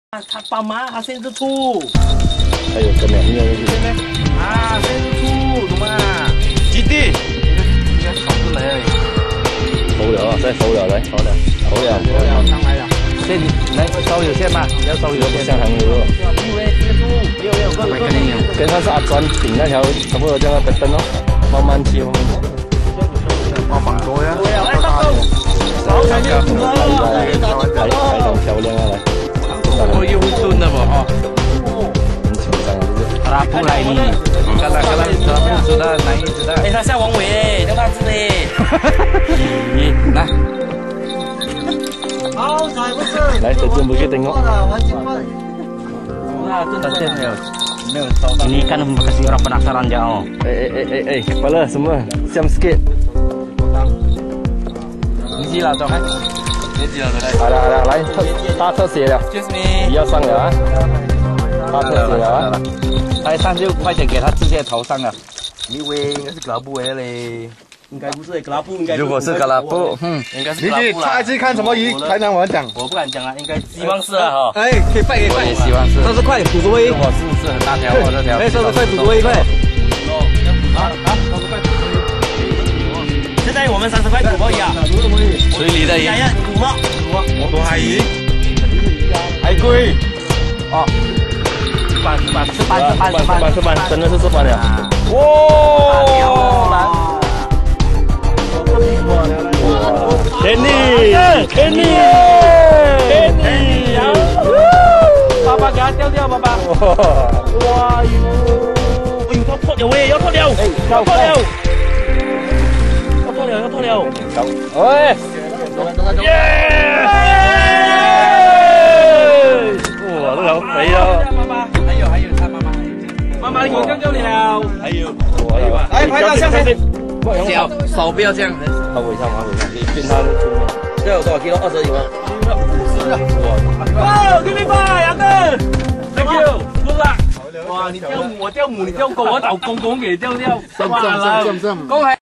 Fymac Fishing Team: 16 kg grouper
the trip's biggest grouper, 16kg (Sarawak Kuching)